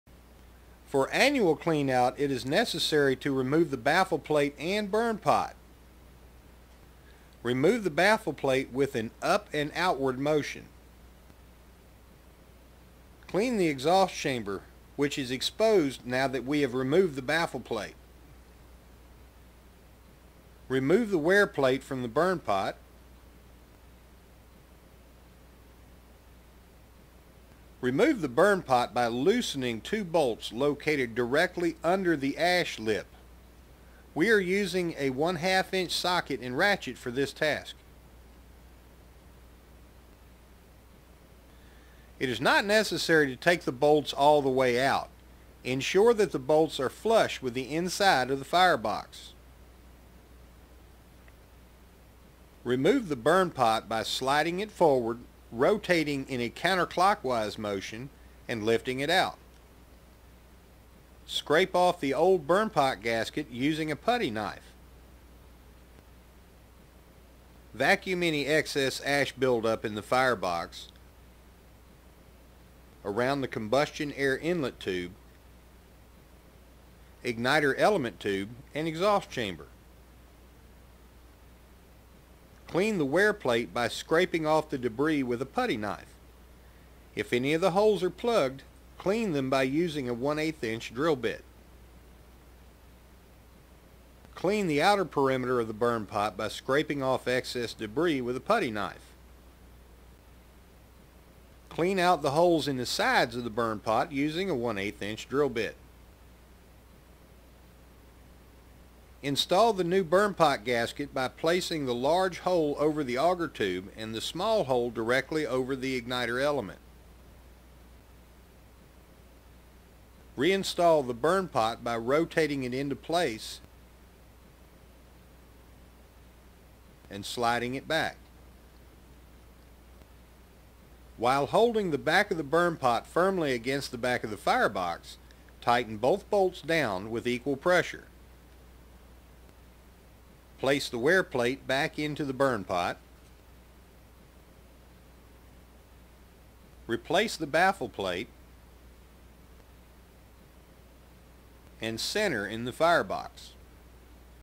AutoStart 08 Annual Clean Out
Cleanout procedures to be performed once per year on the Auto-Start pellet stove from England's Stove Works. Models with 10, 22, PDV or PDVC in model number.
Also shows how to remove & replace the burn pot and the burn pot gasket.